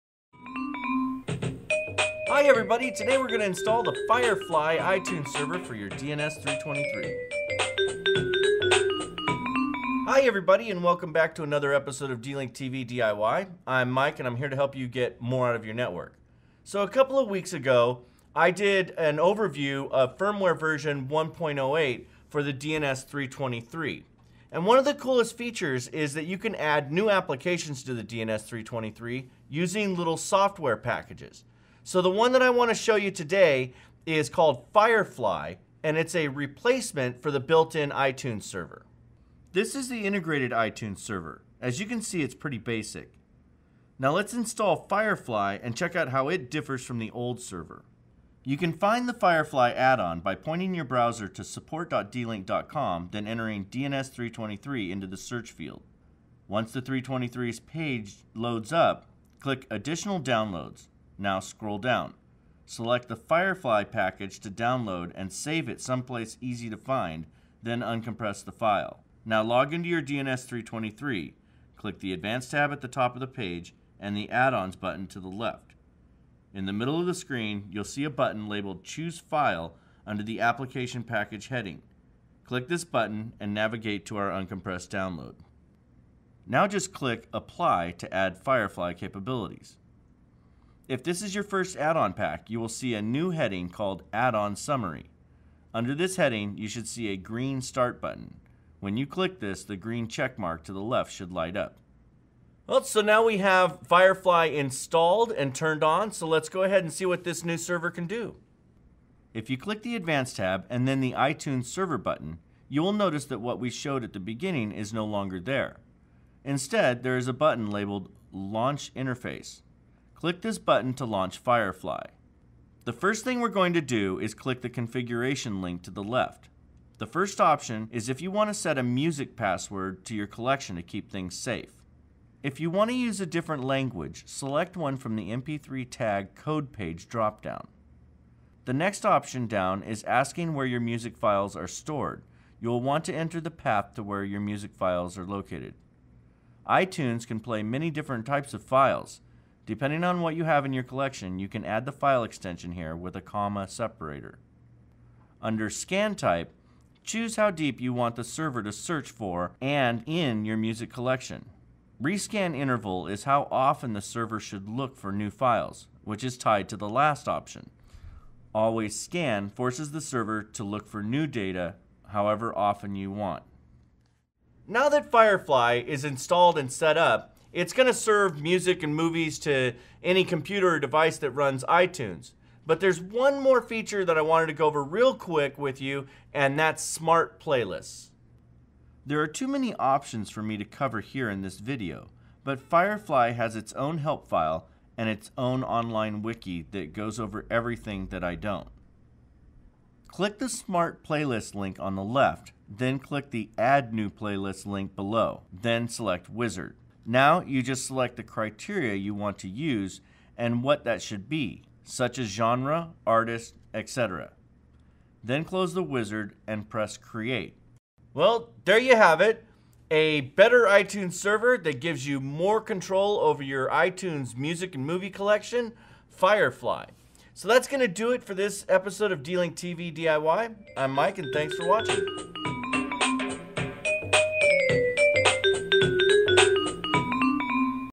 D-LinkTV D.I.Y. "Installing Firefly on the DNS-323"
In this D-LinkTV DIY episode, Mike installs the Firefly iTunes server on the DNS-323.